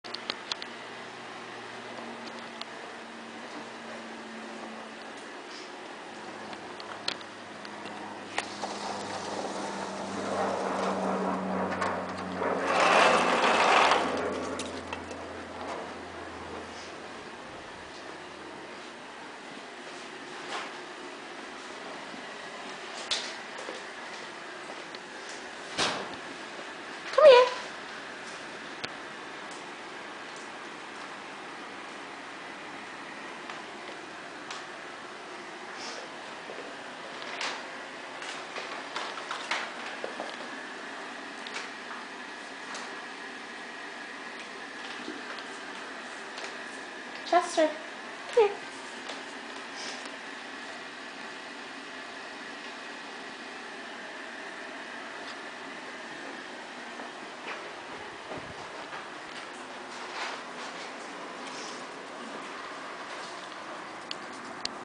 My Pet squirrel Chester who comes inside and eats my food
This little squirrel is so funny! He is some stray squirell - most are, who comes inside to eat the nuts I give him or chips or whatever, and then leaves until he is hungry again. He is cute and lets me pet his bushy tail!